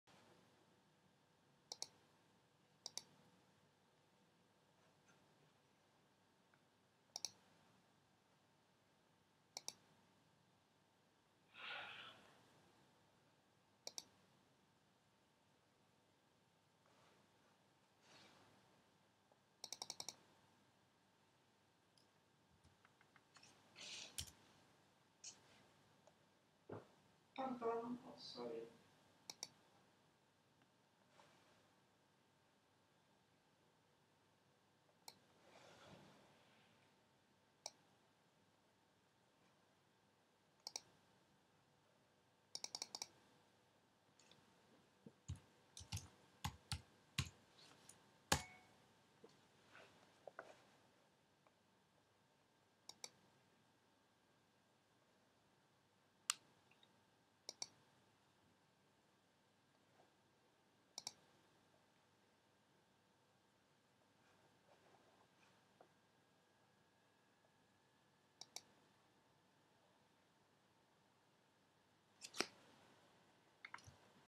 MINT: creating a URL to a digital object in the repository
This video explains how to create a URL to a digital object in the EuropeanaFashion repository. The URL is constructed by a combination of a base url and the filename of the digital object. In this example, the filename was not available in the metadata record. Therefore, it is created from the value of 'inventario', to which the string 'b.jpg' is added.